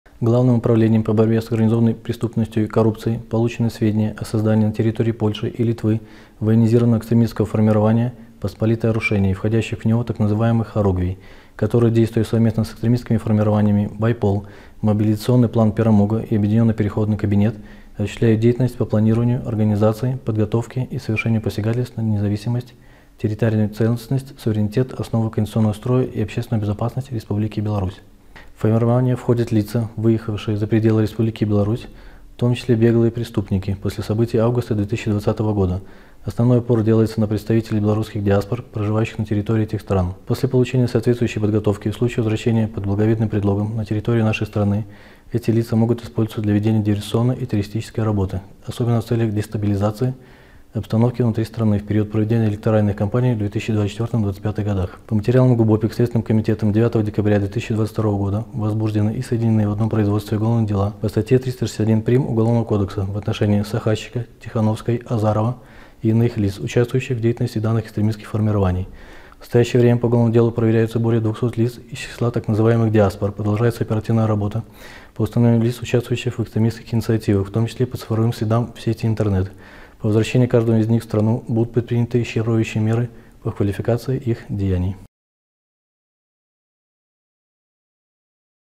⚠️ Новые уголовные дела за создание экстремистского формирования в Польше и Литве
Сотрудниками ГУБОПиК МВД получены сведения о создании в Польше и Литве военизированного экстремистского формирования "Посполитое Рушение" и входящих в него структурных подразделений - "хоругвей".  Совместно с другими экстремистскими формированиями они проводят работу по организации посягательств на территориальную целостность, суверенитет, основы конституционного строя и общественную безопасность Беларуси.

По замыслу создателей, после подготовки участники формирования из числа беглых преступников, представителей диаспор и студентов, обучающихся за рубежом, должны вернуться в Беларусь под благовидными предлогами. В последующем сформировать уже обученную «пятую колонну» для ведения диверсионной, террористической и агентурной работы. Эту организацию планируется использовать в том числе для дестабилизации обстановки внутри страны в период проведения электоральных кампаний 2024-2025 годов.

По материалам ГУБОПиК МВД следователями возбуждены и соединены в одном производстве уголовные дела за создание экстремистского формирования либо участие в нем в отношении Сахащика В.С., Тихановской С.Г., Азарова А.А. и других лиц. В настоящее время по уголовному делу проверяется более 200 человек.
Правоохранителями продолжается оперативная работа по установлению активистов и представителей диаспор, участвующих в экстремистских инициативах, в том числе по цифровым следам в интернете. При возвращении указанных лиц в страну будут предприняты исчерпывающие меры по квалификации их деяний. От ответственности не уйдет никто!
Комментарий начальника отдела ГУБОПиК МВД Евгения Пилецкого⬆️